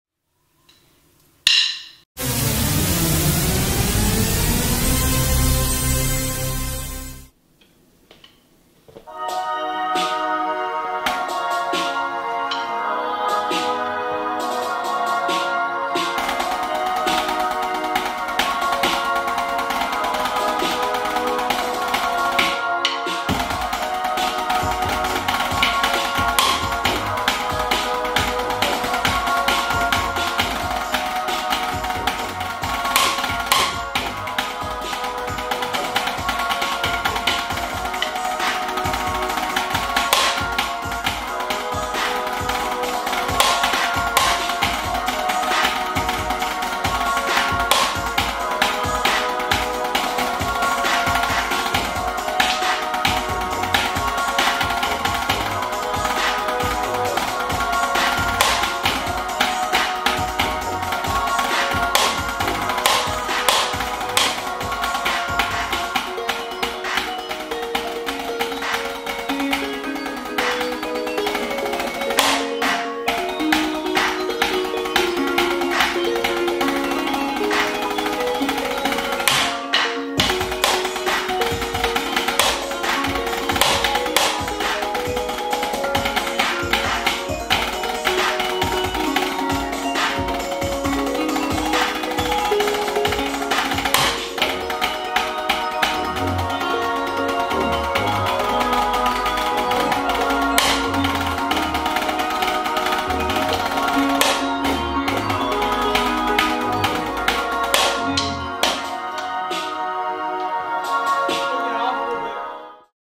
Drum Captain from Atlanta Drum Academy Snare Drum Solo
Snare Drum Solo by Atlanta Drum Academy’s Drum Captain Jasmine Bowens.

Check out Jasmine as she does a quick freestyle to show her skill set on the snare drum with this drum solo. Her hands just flow freely and easily while displaying high level snare drum rudiments . She can play traditional style or even Corp style like DCI, WGI, or The BYOS style . This is her last year with 𝑨𝒕𝒍 Drum Academy and in her high school marching band !!